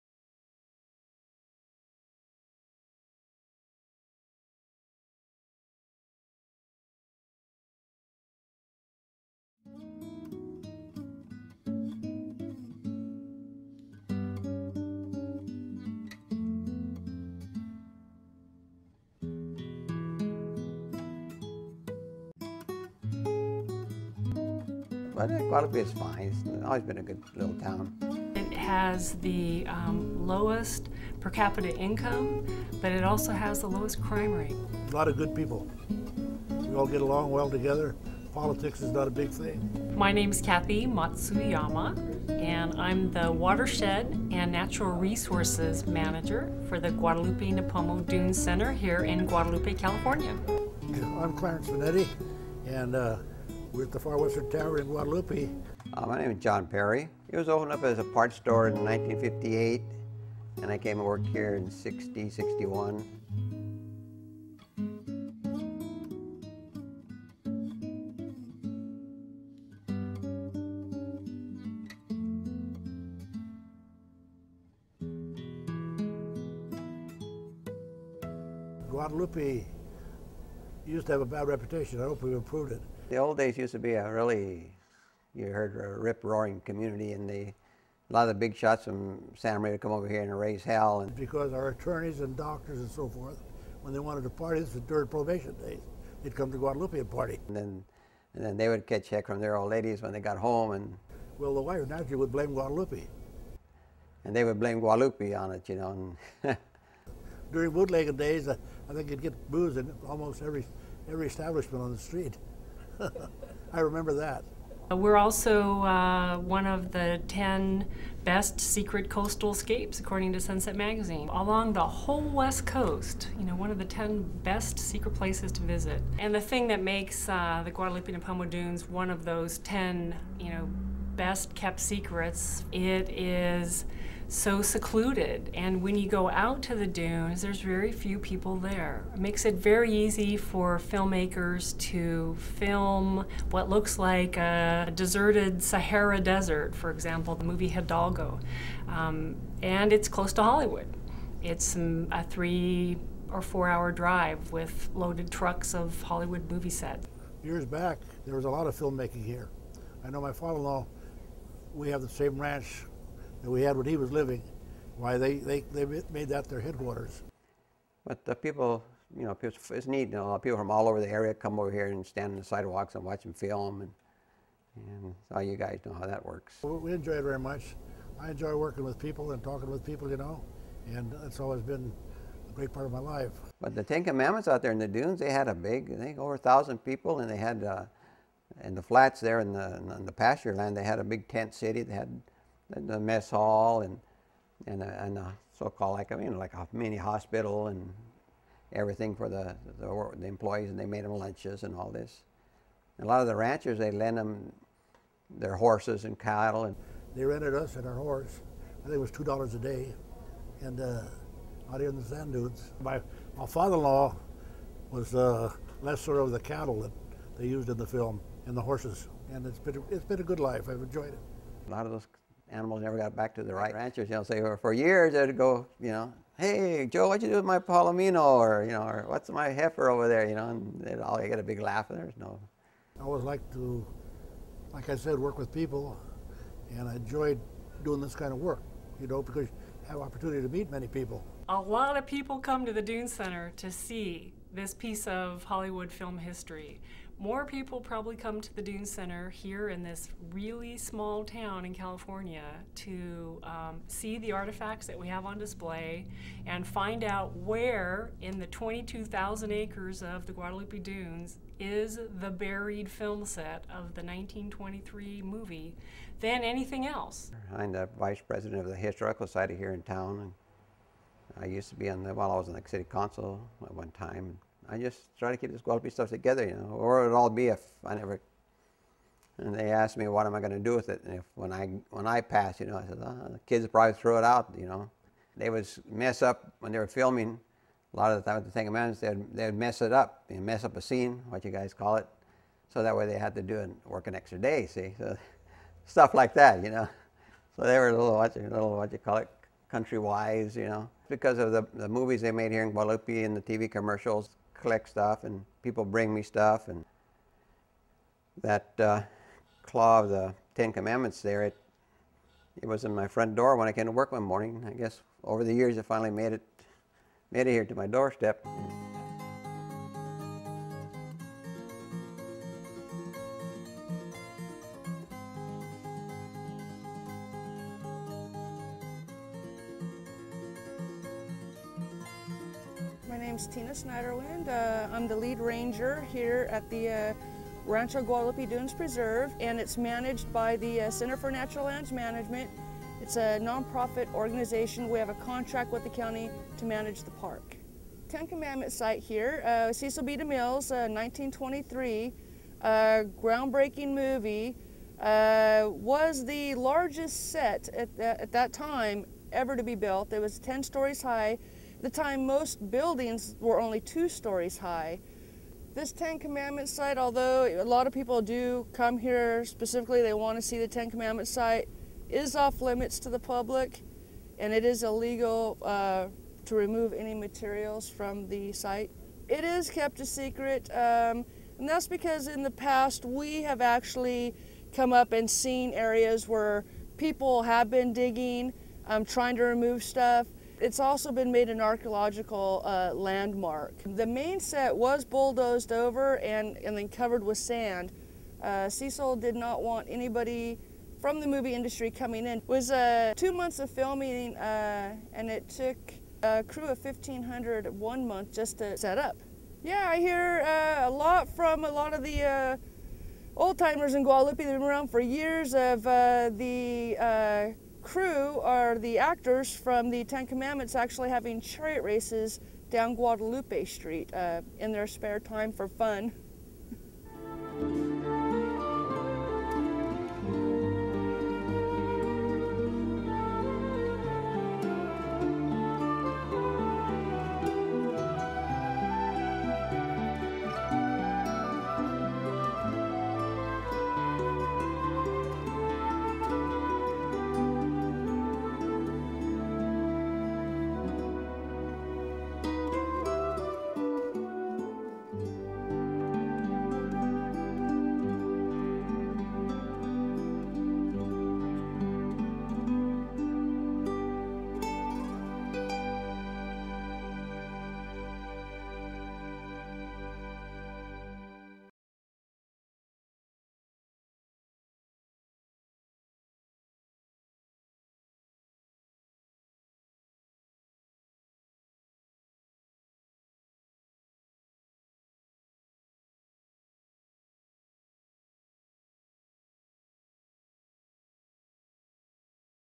Secret In The Sand
Cecil B. Demille buried a huge Egyptian City in the quiet town of Guadalupe Ca. in 1923. This is the story of that towering lost city and the small town where it's buried.